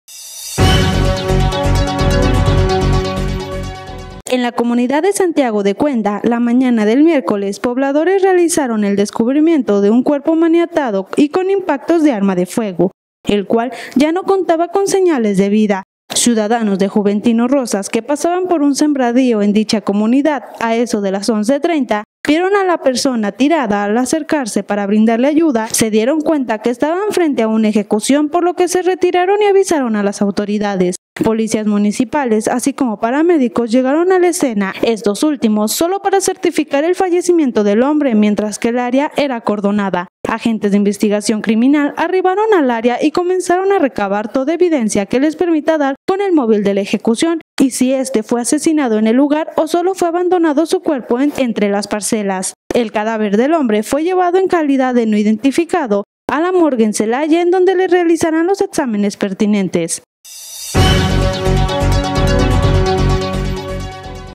Comunidad de Santiago de Cuenda abandonaron el cadáver de un hombre en un sembradío.
Habitantes de la Comunidad de Santiago de Cuenda, en el municipio de JuventinoRosas, localizaron el Cadáver de un hombre Maniatado y con huellas de violencia.